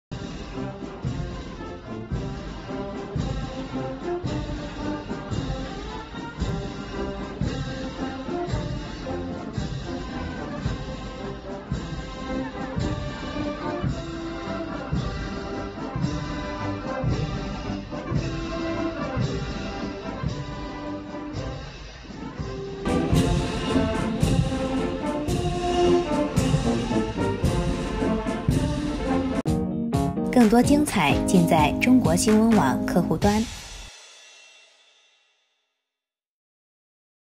阅兵大典即将开始 军乐团在现场排练
10月1日，在北京天安门广场，阅兵大典即将开始，军乐团在现场进行排练。